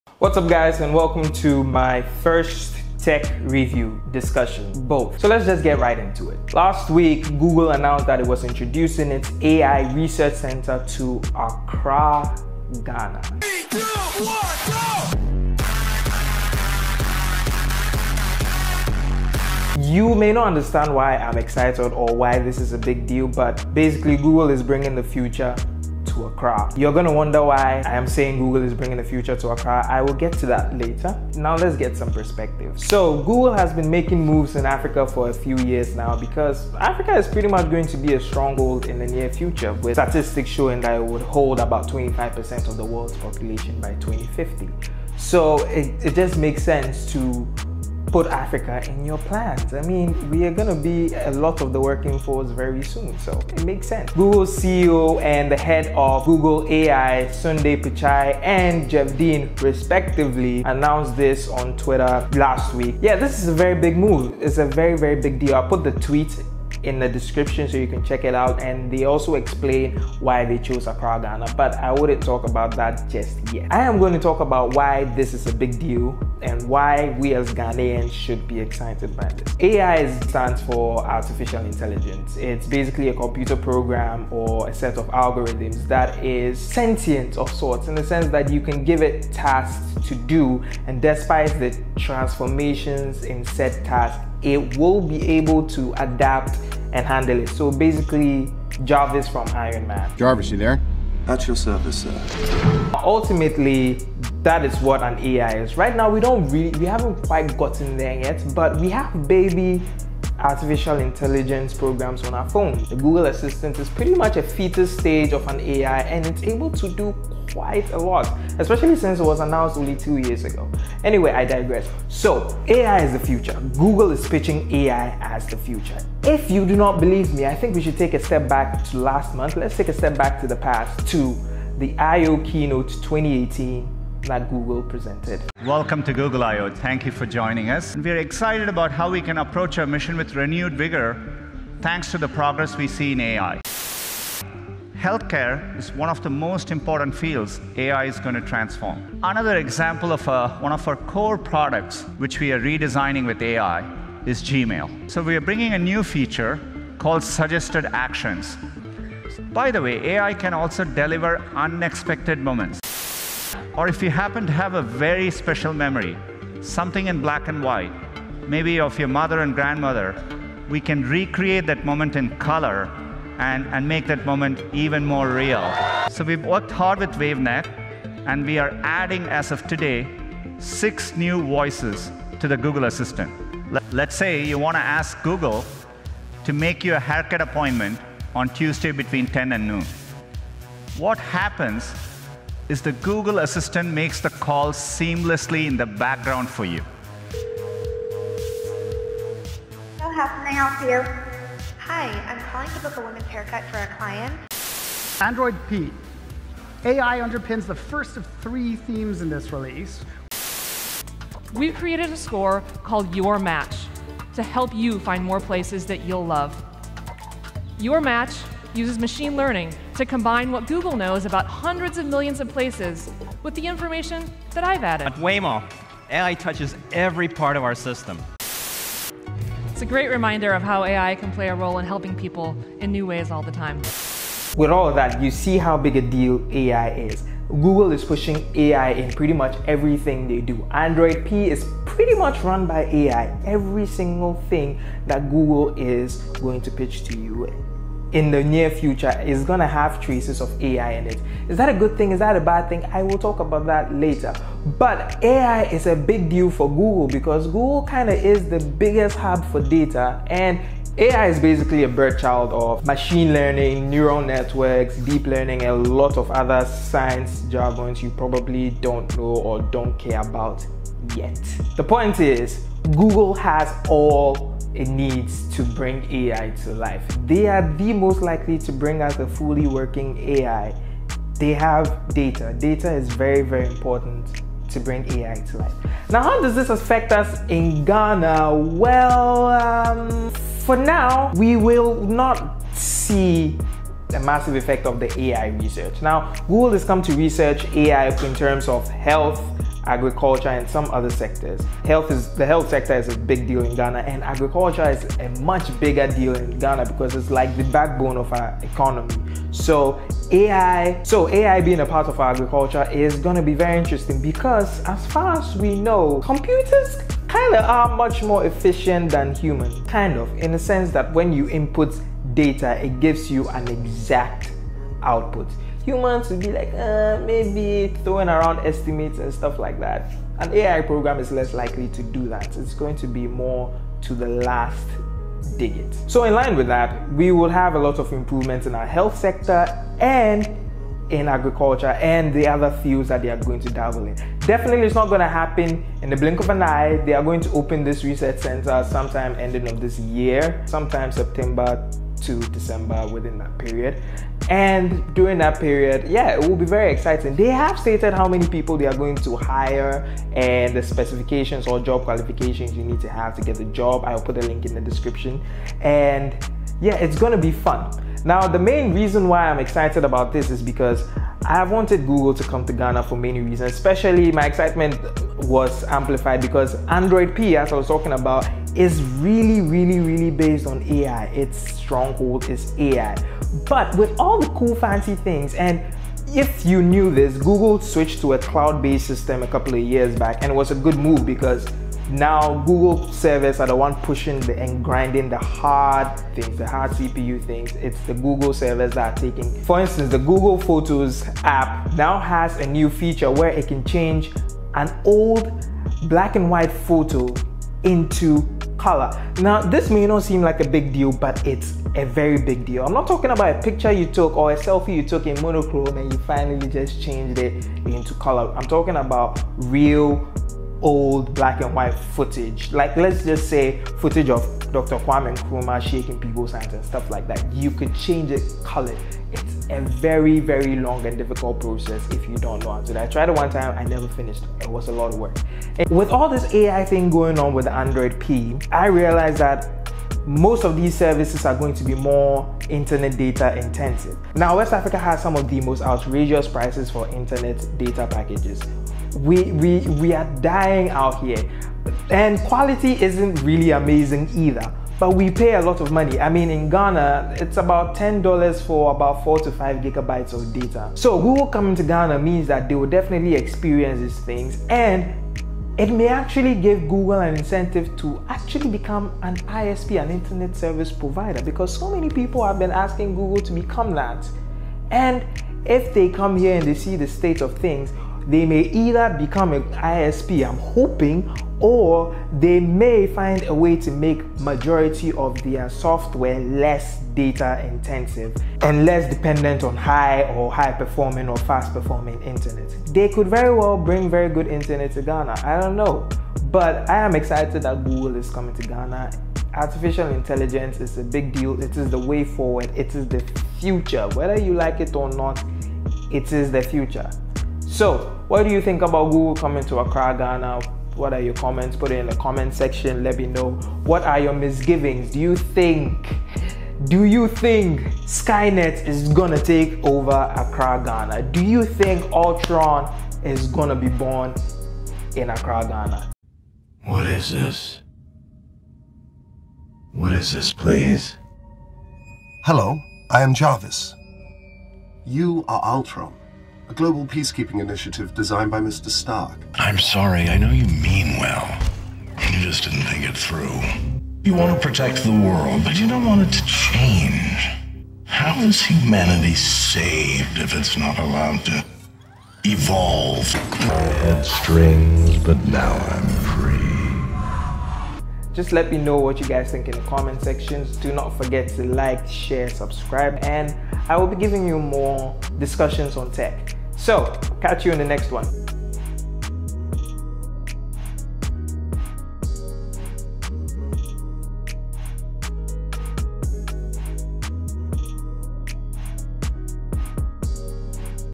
A Ghanaian Reviews; Google Comes to Ghana
Google has announced its plans to set up an A.I Research Institute in Accra, Ghana later this year. As a Ghanaian and a fan of technology, this is some EXCITING news. Why am i excited? Why should you be excited? Allow me to tell you why.
P.S: I shot this a bit earlier and only got round to uploading it, my bad.